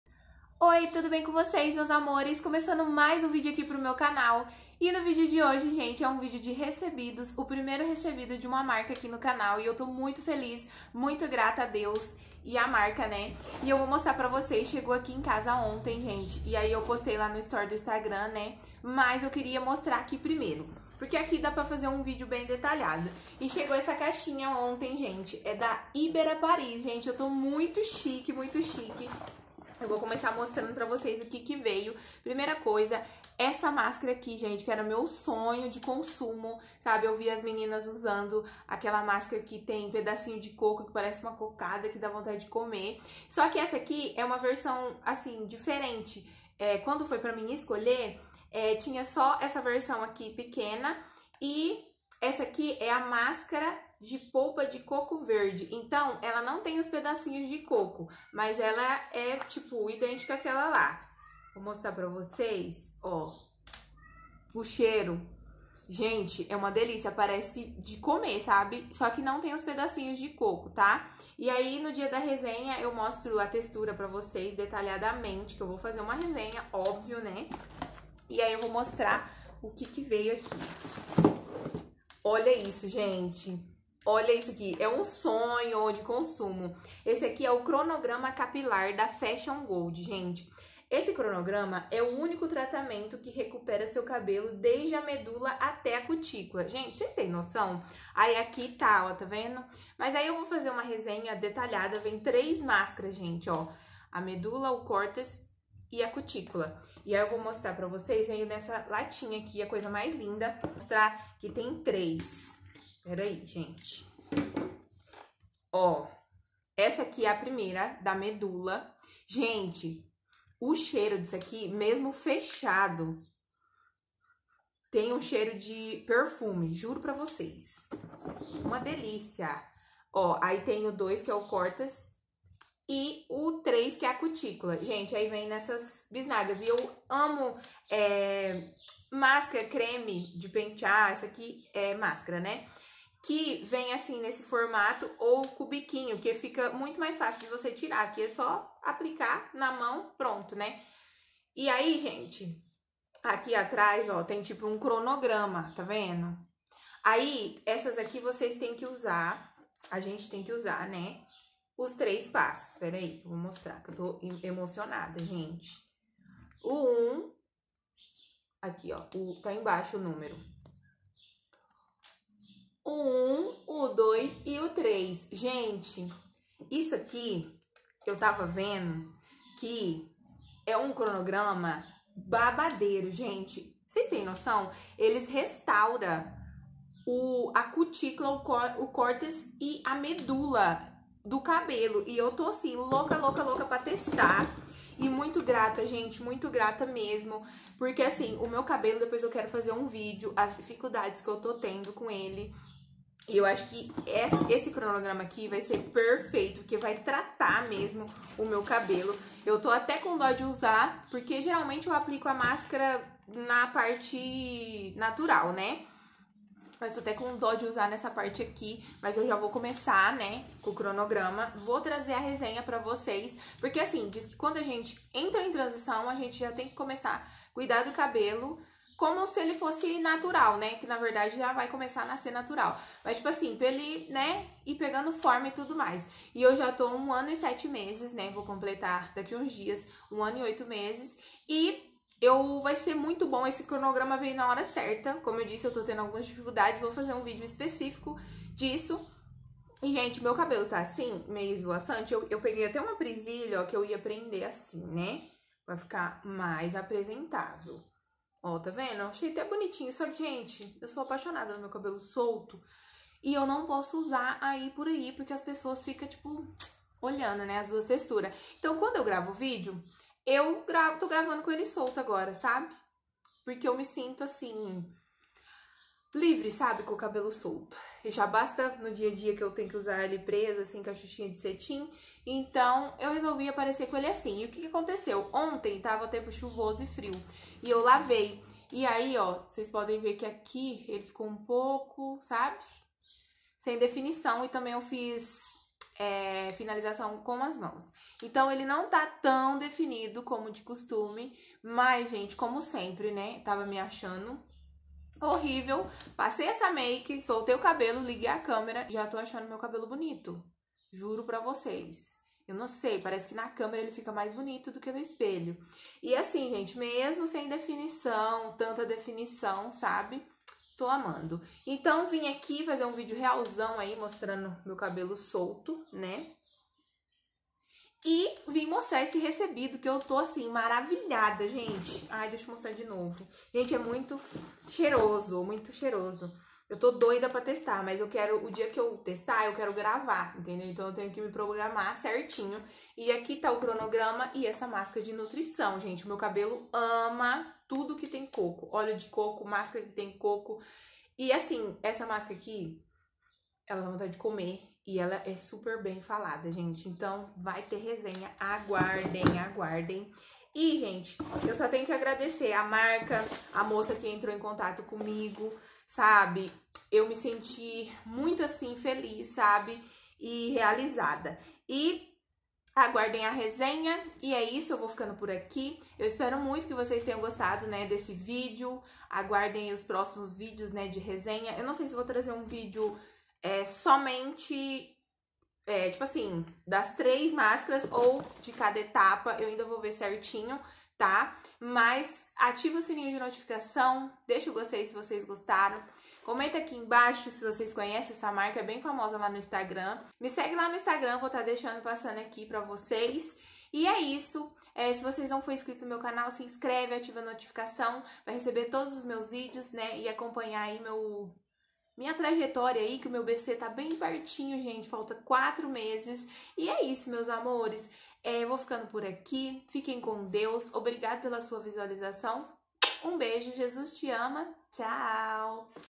RECEBIDOS QUE FELICIDADE🙏🏽
Oi tudo bem com vocês meus amores ,no vídeo de hoje vou mostrar os recebidos e espero muito que vocês gostem.